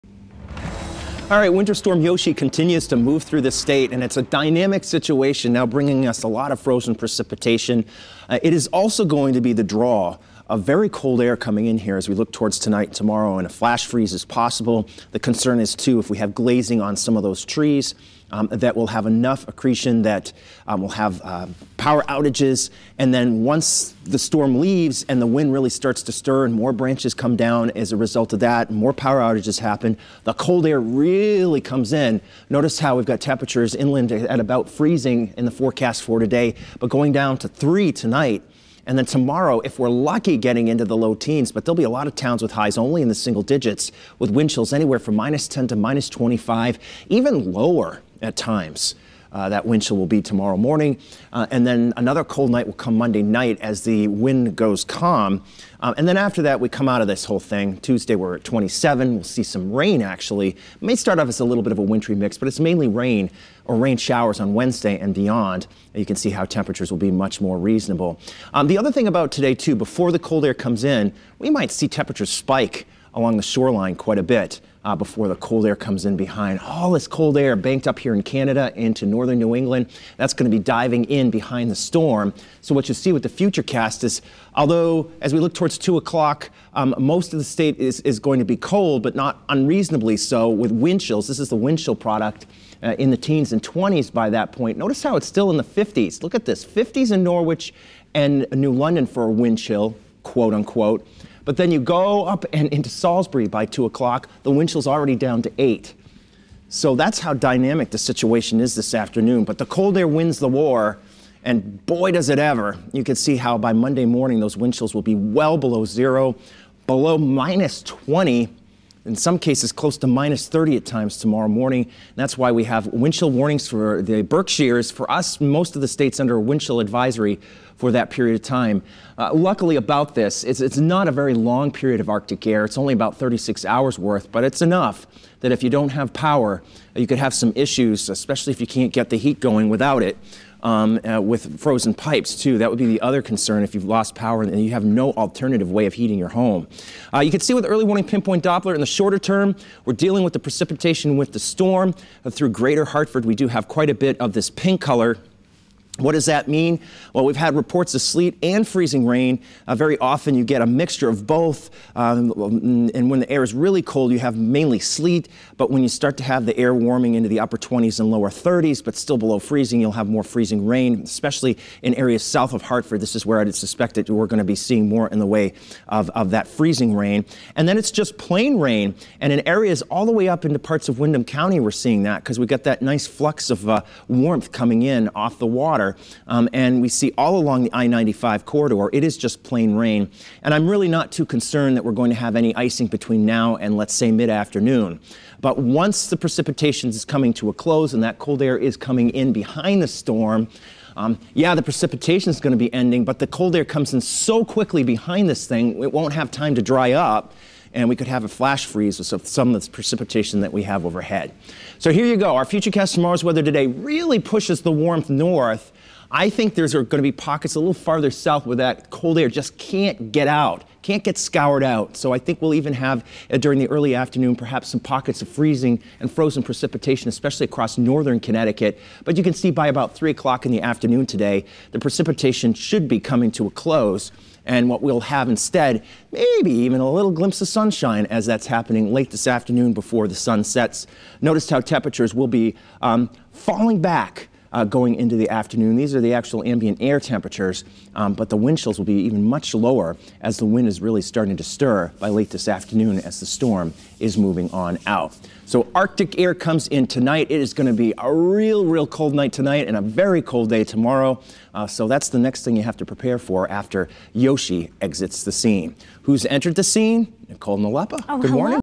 FORECAST: Snow transitioned to wintry mix, rain Sunday morning
Snow from Winter Storm Yoshi turned to a wintry mix and rain Sunday morning.